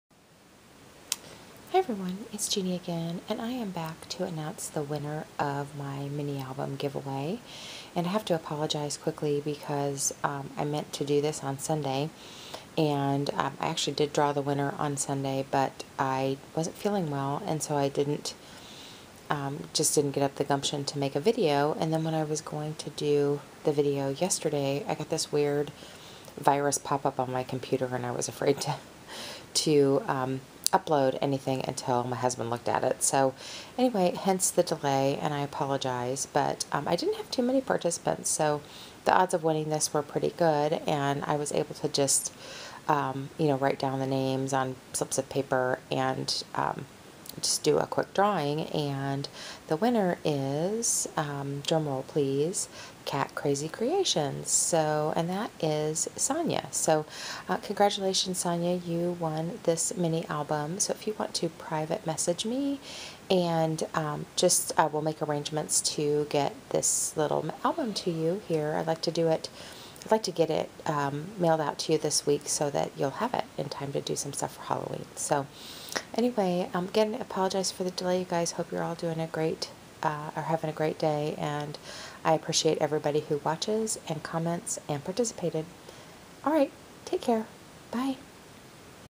Mini Album Winner Announced
Congratulations to catcrazycreations for winning the Halloween mini album!